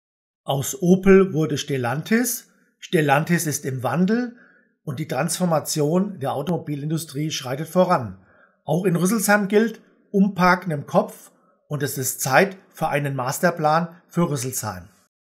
Masterplan für Rüsselsheim
Opel verläßt das Adam Opel Haus am Rugbyring udn zieht mit der Hauptverwaltung ins K 40. Laut Oberbürgermeister Udo Bausch (Hufa am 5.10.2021) werden zukünftig große Werksareale frei. Jetzt gilt auch für die Stadtpolitik "Umparken im Kopf".

Statt in "KLEINTEILIGKEIT" zu denken ist es Zeit für einen MasterplanRüsselsheim. Da muss jetzt nichts über das Knie gebrochen werden. Aber aus der Stadtpolitik muss ein Zeichen kommen, dass man aus dem IKEA-Desaster gelernt hat.